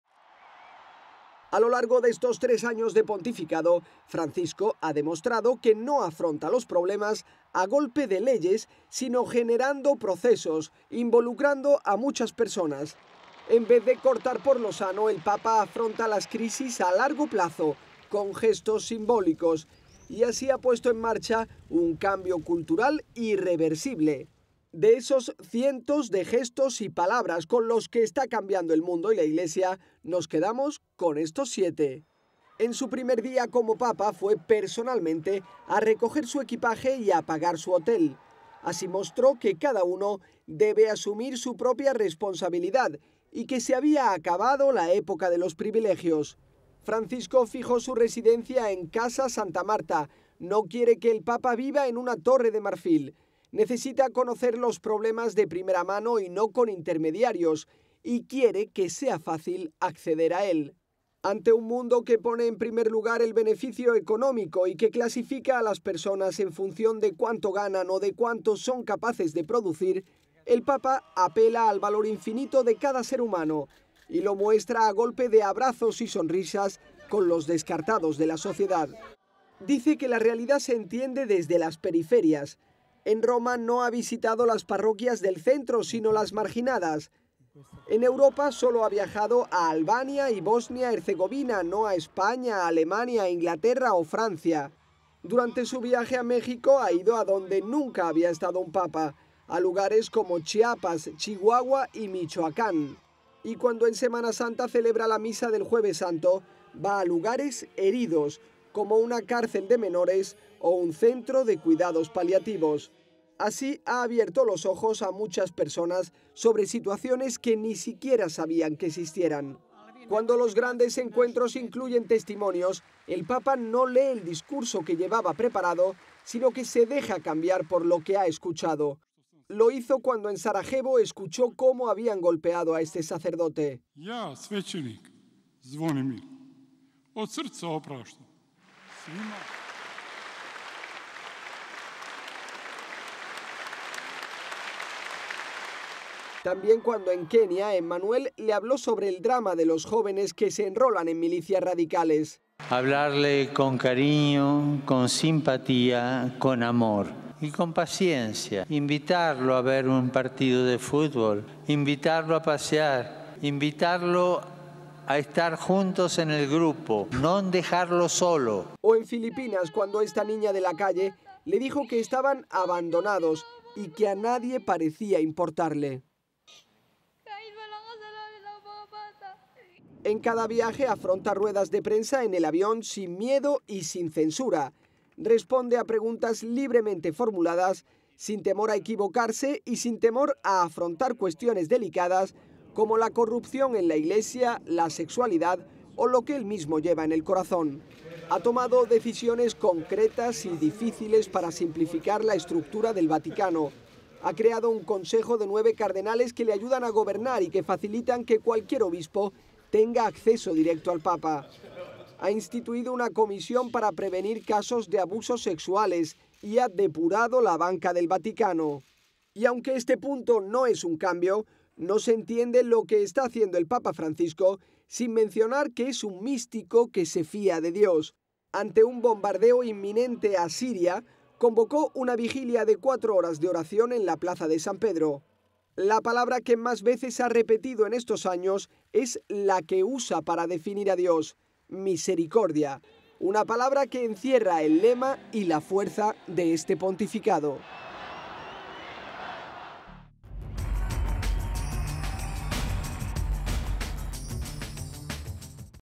Los siete grandes cambios que ha realizado el Papa Francisco
Con ellos, en tres años de Pontificado ha puesto en marcha un profundo cambio cultural.